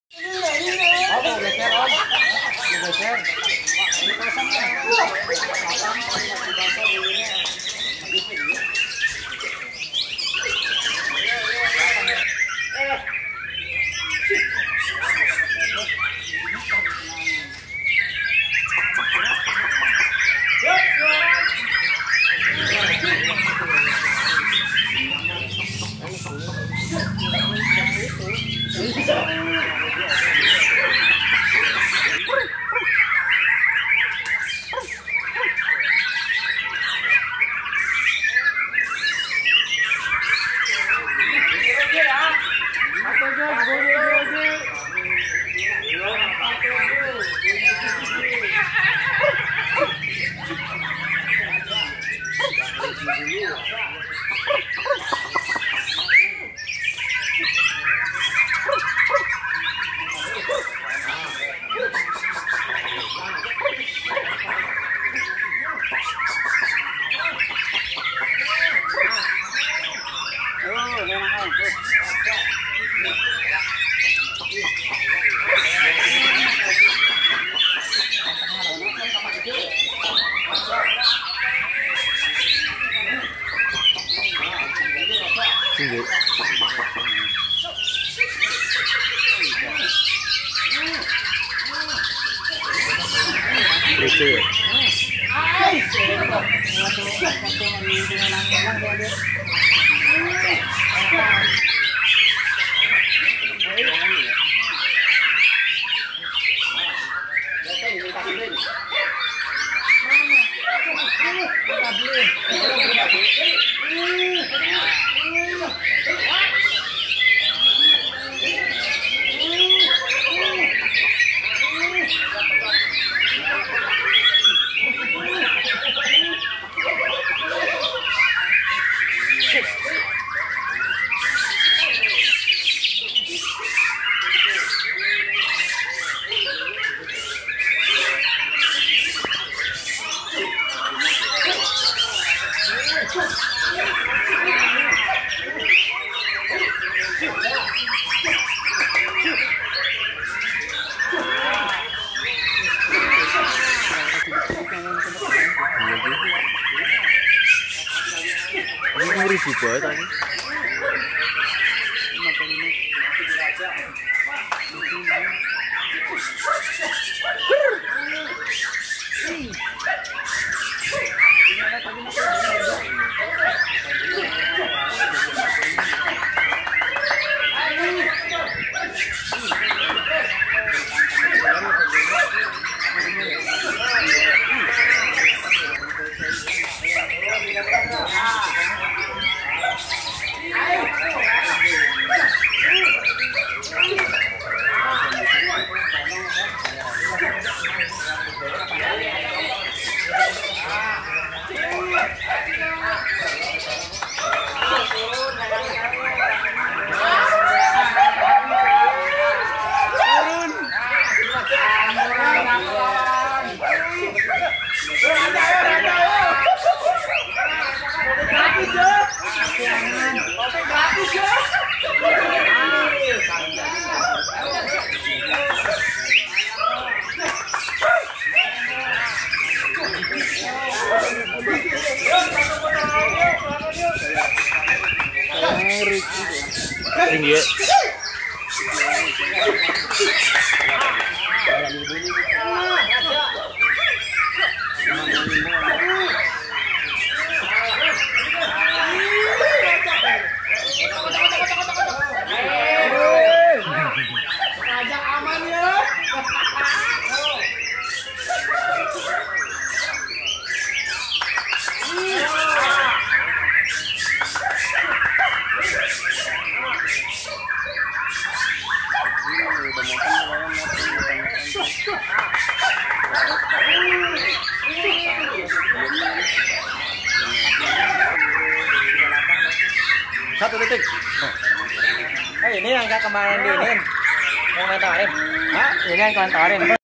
KESERUAN GANTANGAN MURAI BATU ✔️ DIKOTA KARAWANG
Assamualaikum wr wb
Kali ini @muraiindo akan membahas atau menampilkan perlombaan murai batu di kota karawang khususnya babaton

Silakan menyimak video nya.